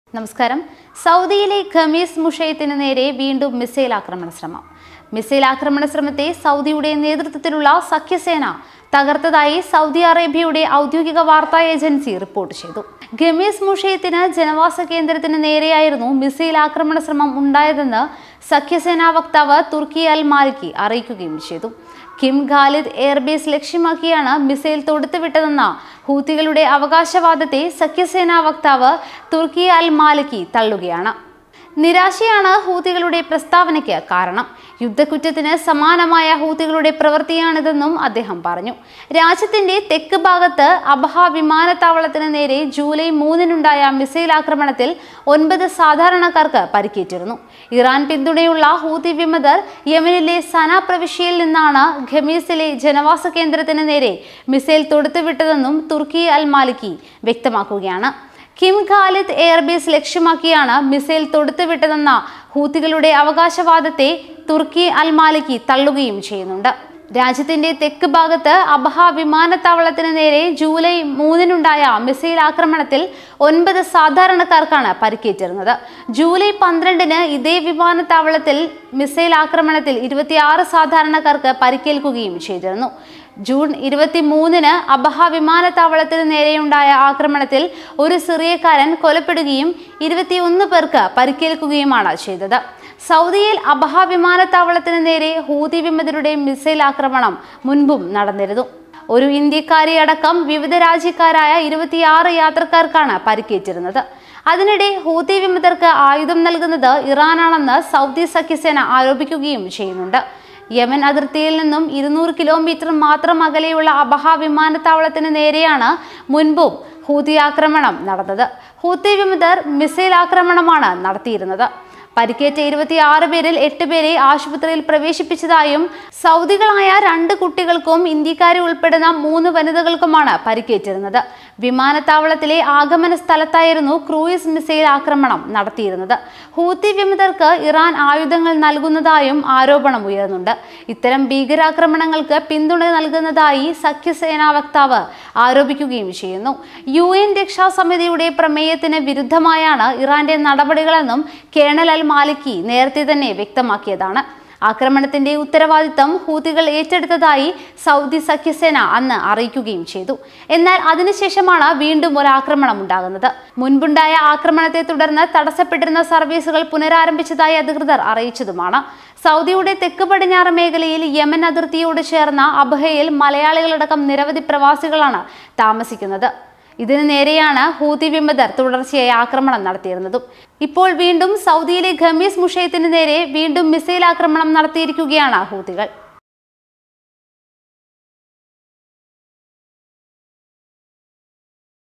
സൗദി ജനവാസ കേന്ദ്രത്തിനുനേരെ മിസൈല്‍ I saudi arabia
സൗദിയിലെ ഖമീസ് മുഷൈത്തിനു നേരെ മിസൈല്‍ ആക്രമണ ശ്രമം. മിസൈല്‍ ആക്രമണശ്രമത്തെ സൗദിയുടെ നേതൃത്വത്തിലുള്ള സഖ്യസേന തകര്‍ത്തതായി സൗദി അറേബ്യയുടെ ഔദ്യോഗികവാര്‍ത്ത ഏജന്‍സി റിപ്പോര്‍ട്ട് ചെയ്തു.